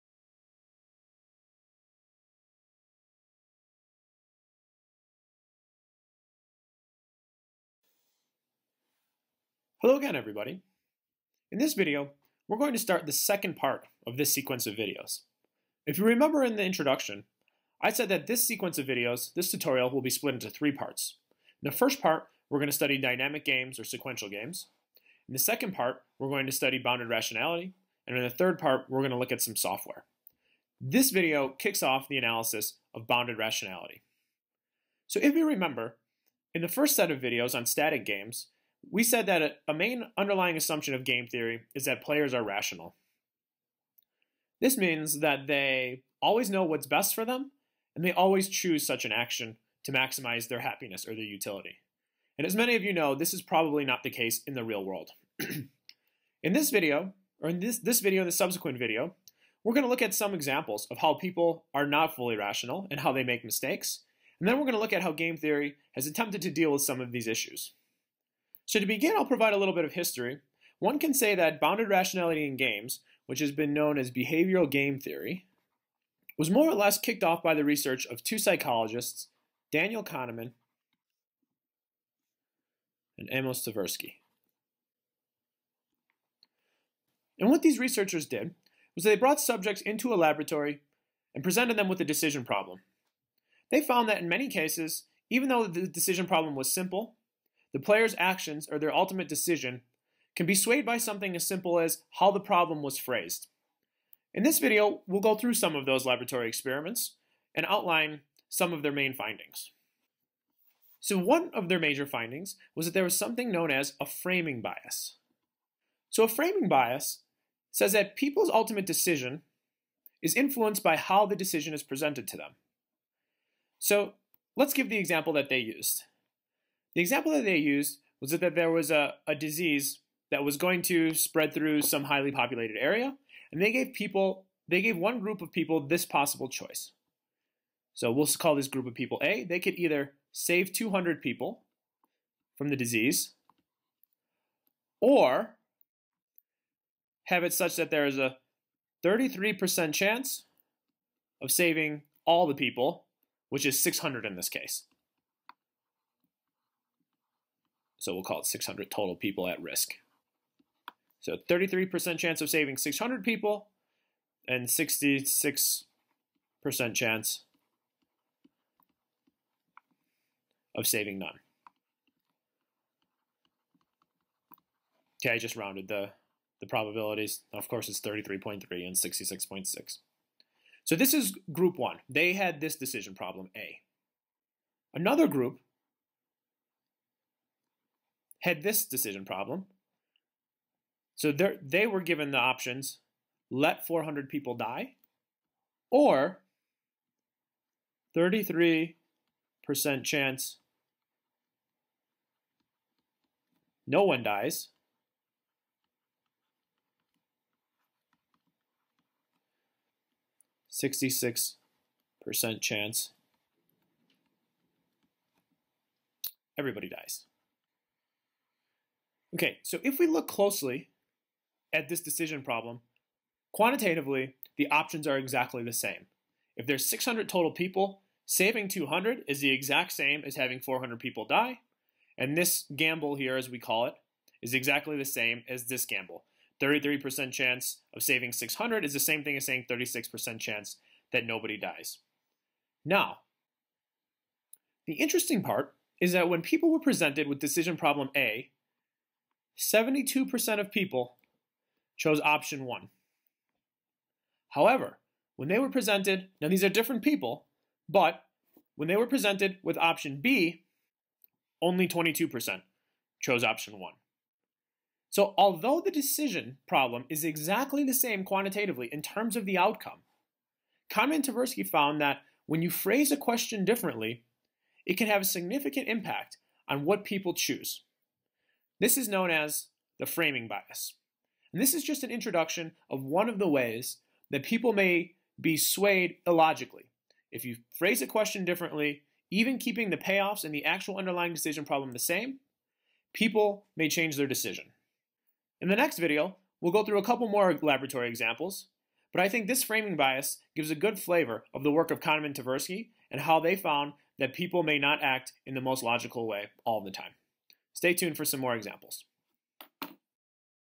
Game Theory Tutorial: Dynamic Games: Framing Biases
Game theory is the standard quantitative tool for analyzing the interactions of multiple decision makers. Its applications extend to economics, biology, engineering and even cyber security. Furthermore, many complex systems involve multiple decision makers and thus a full analysis of such systems necessitates the tools of game theory.

This tutorial is the second part of of Dr. Justin Grana's tutorial on game theory from Complexity Explorer. The tutorial is divided into three parts: Dynamic and sequential games with discusses extensive-form representation. The second part of the series discusses bounded rationality and imperfect decision making and information, both in theoretical models and in laboratory settings. Finally we look at Gambit, a software program used by theoreticians to solve games.